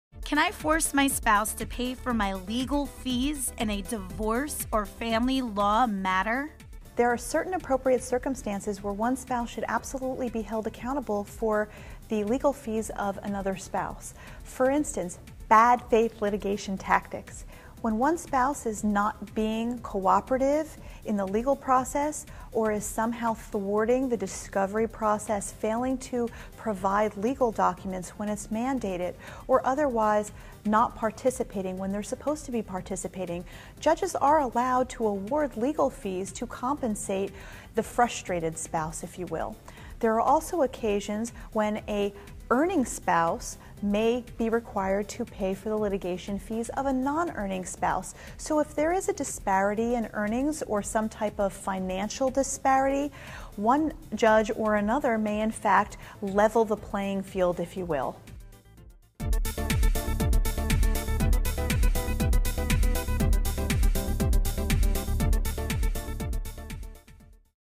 FAQ Can I force my spouse to pay for my legal fees in a divorce or family law matter?
VIDEO TRANSCRIPT
There are certain appropriate circumstances where one spouse should absolutely be held accountable for the legal fees of another spouse. For instance, bad faith litigation tactics. When one spouse is not being cooperative in the legal process, or is somehow thwarting the discovery process, failing to provide legal documents when it's mandated, or otherwise not participating when they're supposed to be participating, judges are allowed to award legal fees to compensate the frustrated spouse, if you will. There are also occasions when an earning spouse may be required to pay for the litigation fees of a non-earning spouse. So, if there is a disparity in earnings or some type of financial disparity, one judge or another may, in fact, level the playing field, if you will.